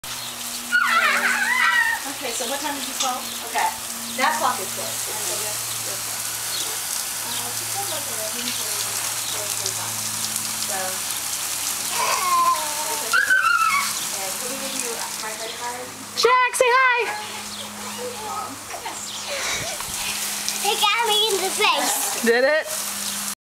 Playing at Jack & Claire's house
July 2011 - Charlie ran through the sprinkler with Claire at the Mancuso house.